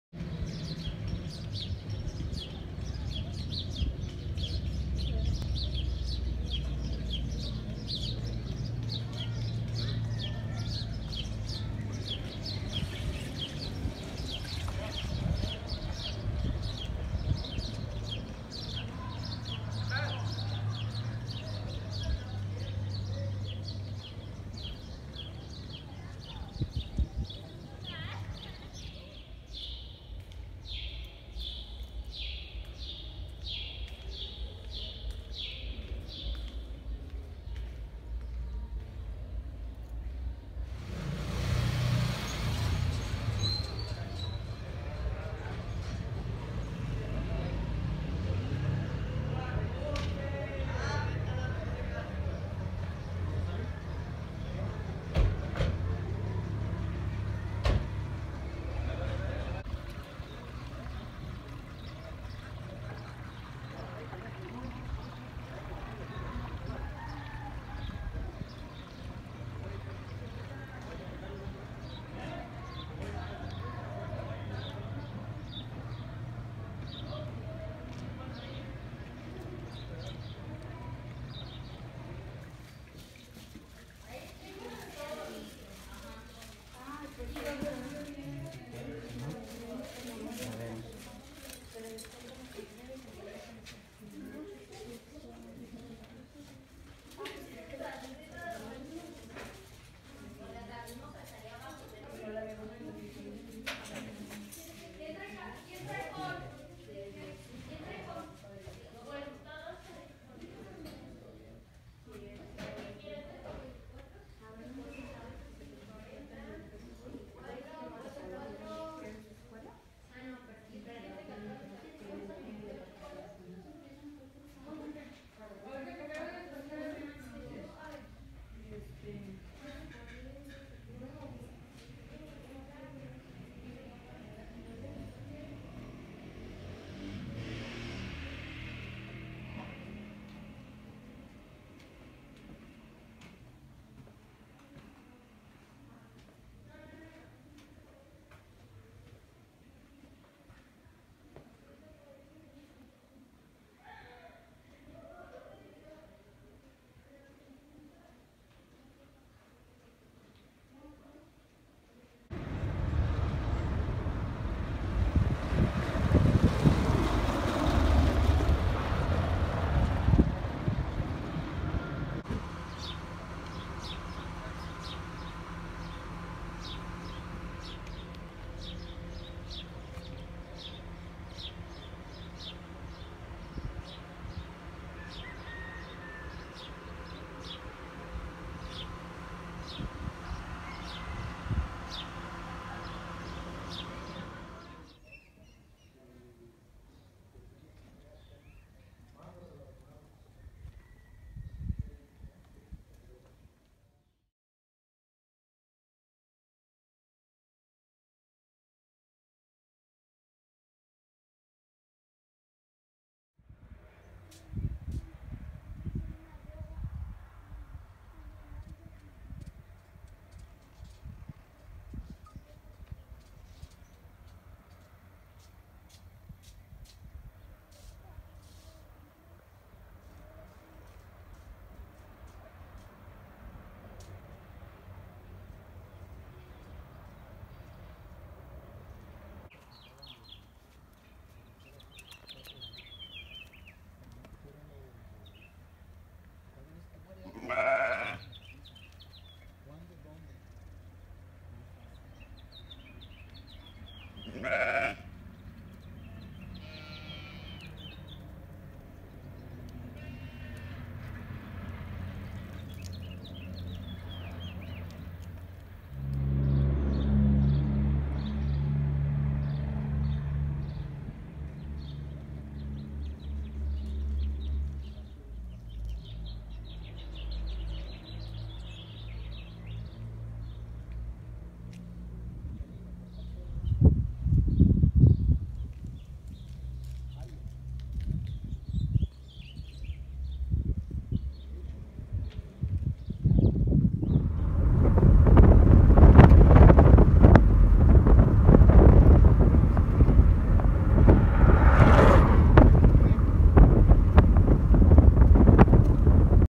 Ruta de los murmullos: lugares de la infancia de Juan Rulfo
Conoce algunos lugares de la infancia de Juan Rulfo y lee el artículo completo en nuestro blog